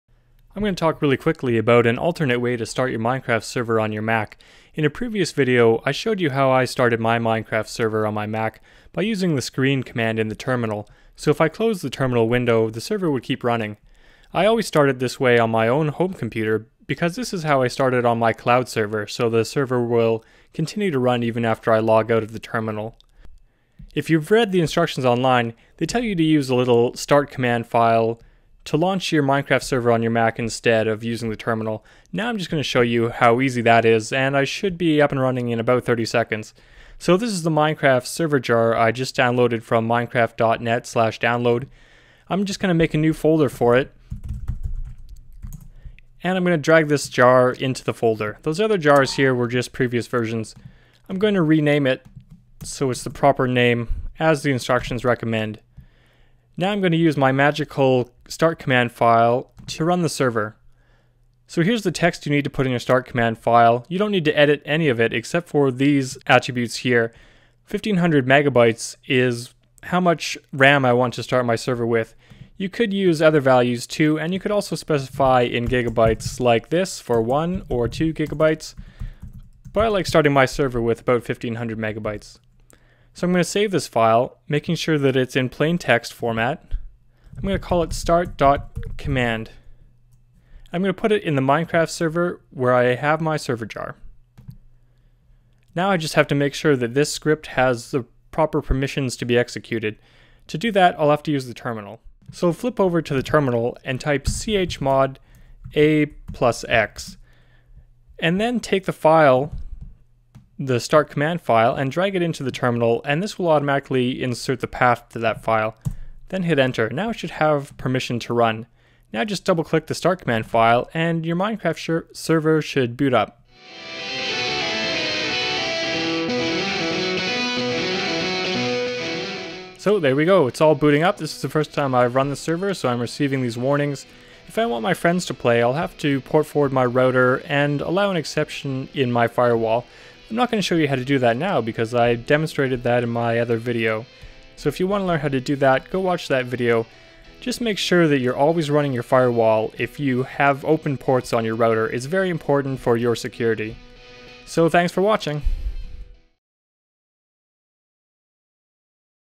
Server in 90 seconds - Minecraft on Mac - start.command
Start a Minecraft server on your Mac OS computer in less than 90 seconds using a start.command file.

This is an easier and nicer method for starting a server at home than using terminal as I did in my previous video.

Here are the secret details that you need to paste into the start.command file:

#!/bin/bash
cd "$(dirname "$0")"
exec java -Xmx1500M -Xms1500M -jar minecraft_server.jar

You can edit the parameters of -Xmx and -Xms to change the amount of RAM the server may use.  It can also be specified in Gigabytes like this: -Xmx1G.  One to two gigabytes work well for small servers.  You don't need too much RAM because your home internet connection will probably limit the maximum number of players you can have before your RAM does.

Minecraft is a really fun game by Mojang AB.